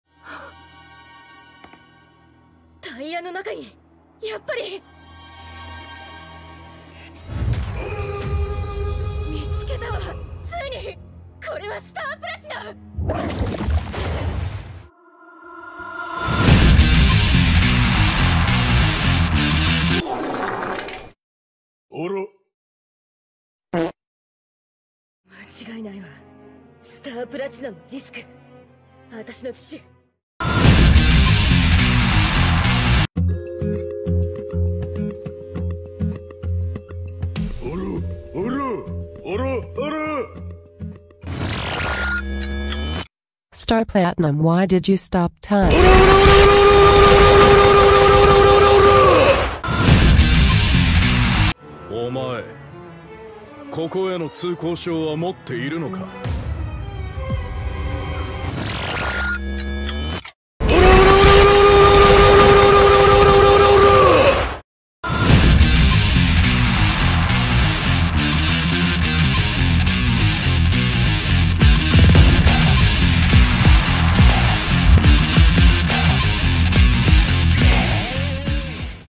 If Star Platinum Didn't Reject Jolyne...
This is what would've happened if Star Platinum didn't eject himself from Jolyne.